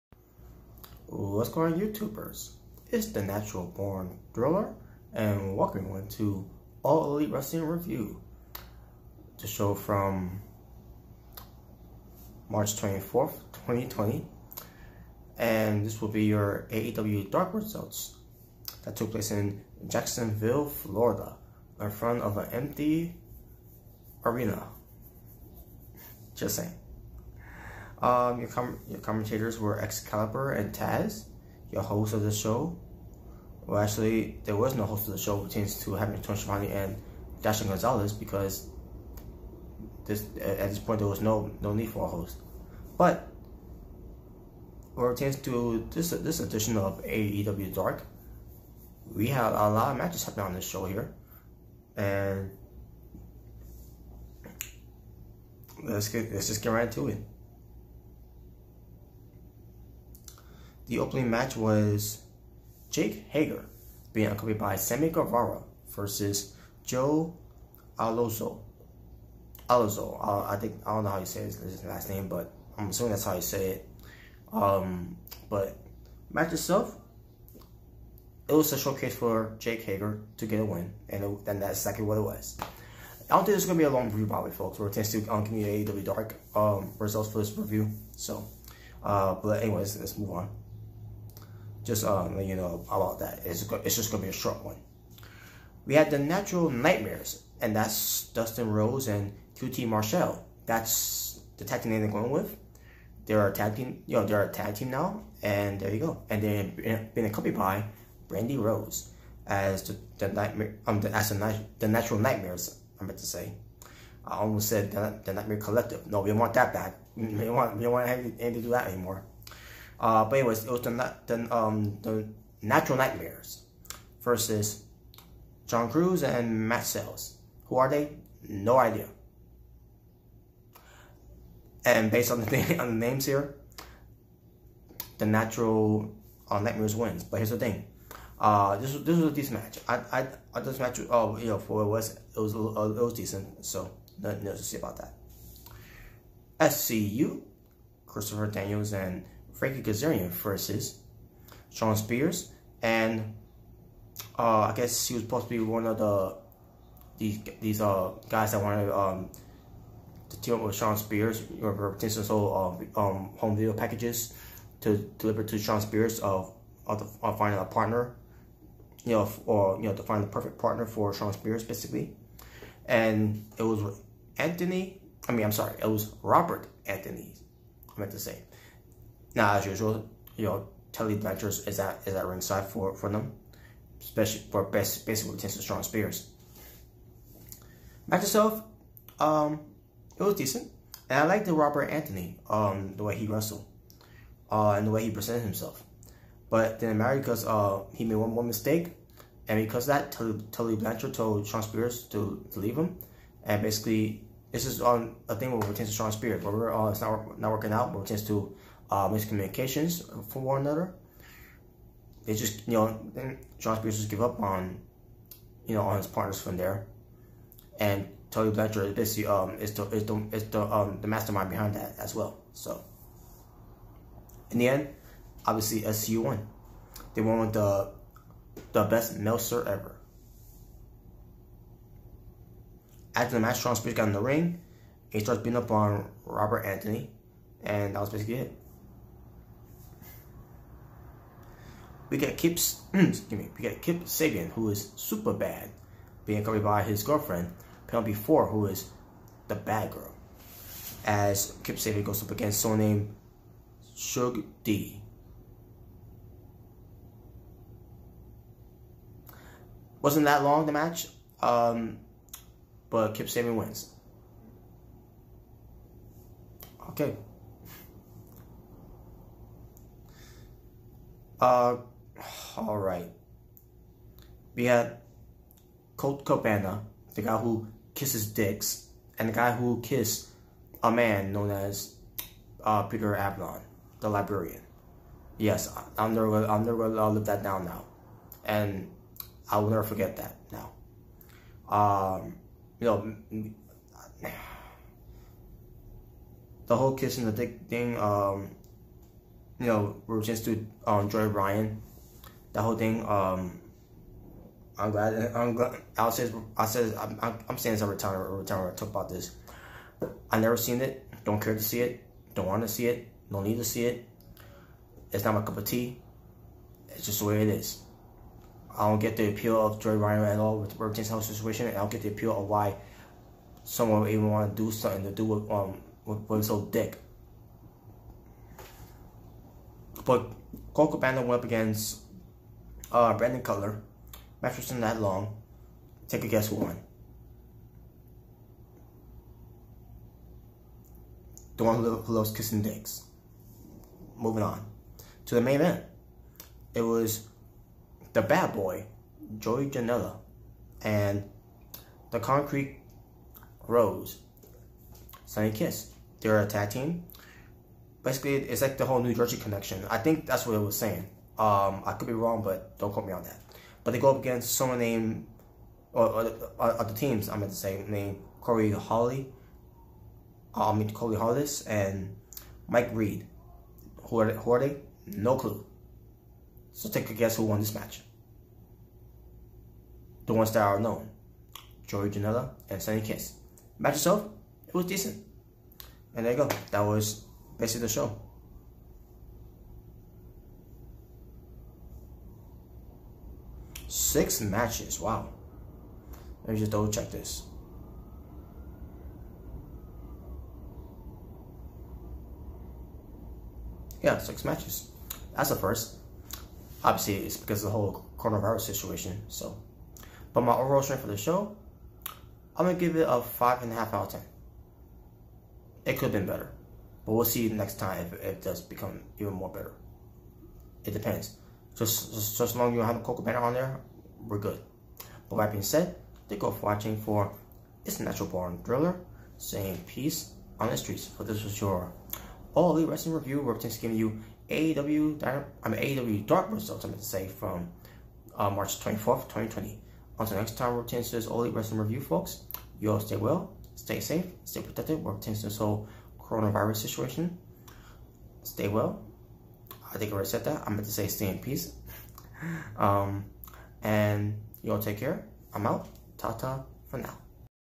ALL ELITE WRESTLING REVIEW | AEW Dark Results/Jake Hager, Natural Nightmares, Robert Anthony? & More
It's the Natural Born Thriller. Welcome to the ALL ELITE WRESTLING REVIEW the show from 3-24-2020. AEW Dark quick results, thoughts, analysis, rants and if you don't like it, Beat It because no one wants to be Defeated.